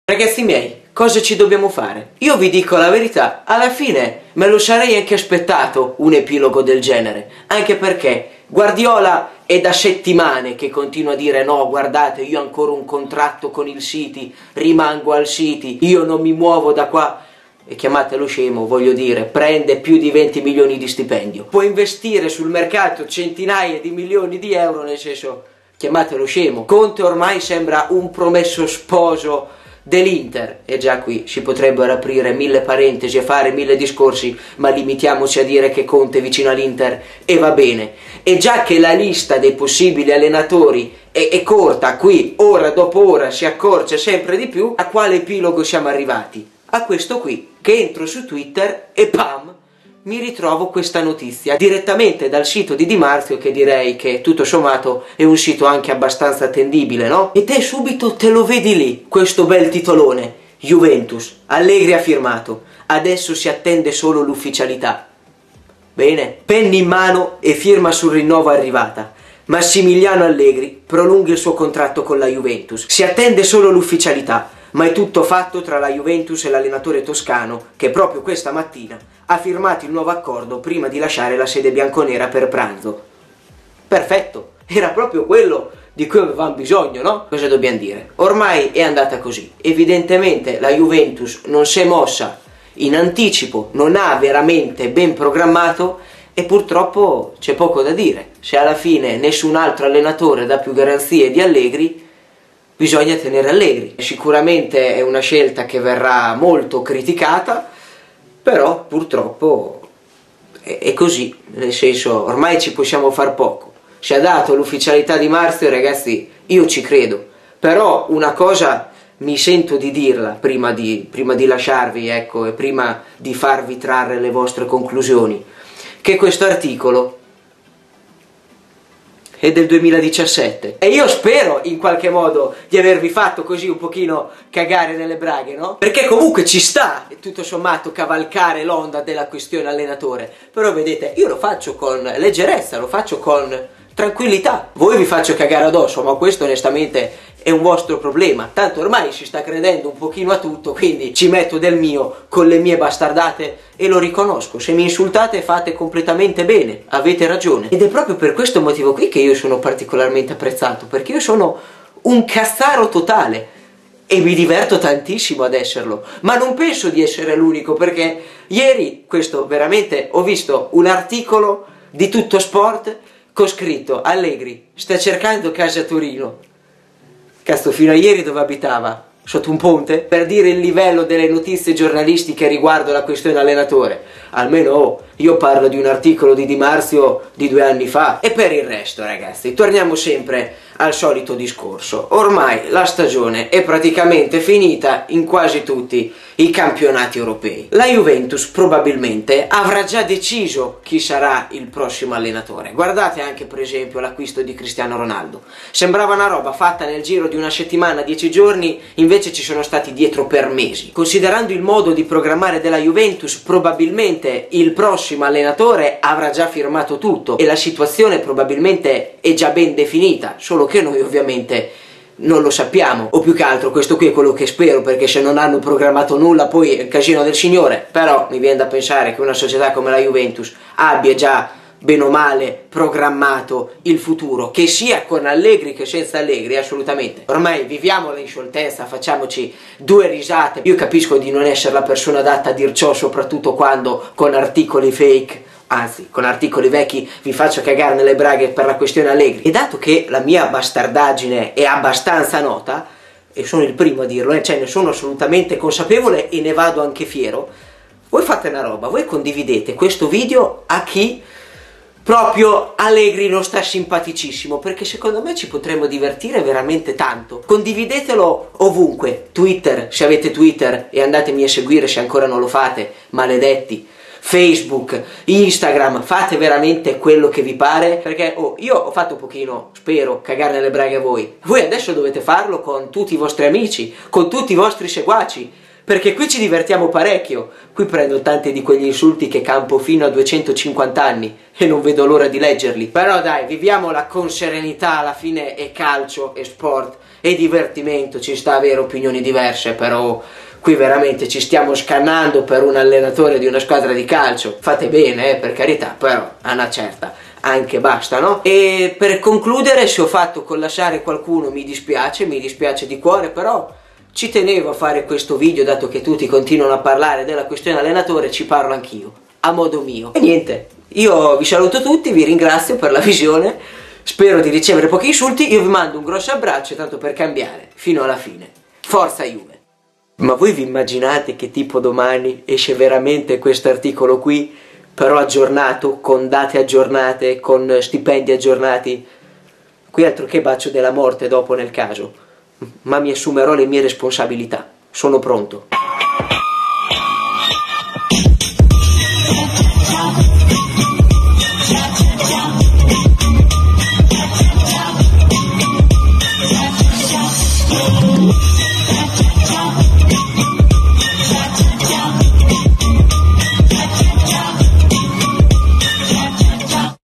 [UFFICIALE] ALLEGRI HA FIRMATO IL RINNOVO CON LA JUVENTUS!
Era solamente questione di giorni e la notizia, dopo tanta attesa, è arrivata. 😓😓😓

Instragram: @kinoshionyt